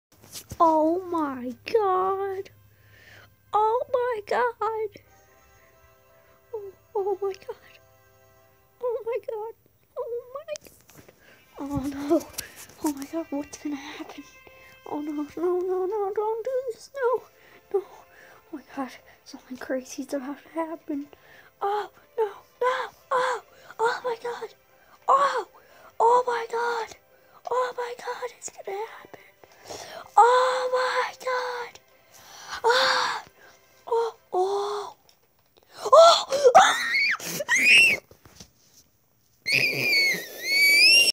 Jaden Meets A Falling Waffle!
All i do is scream and yell "OH MY GOD!!! OH OH!" the entire time lol.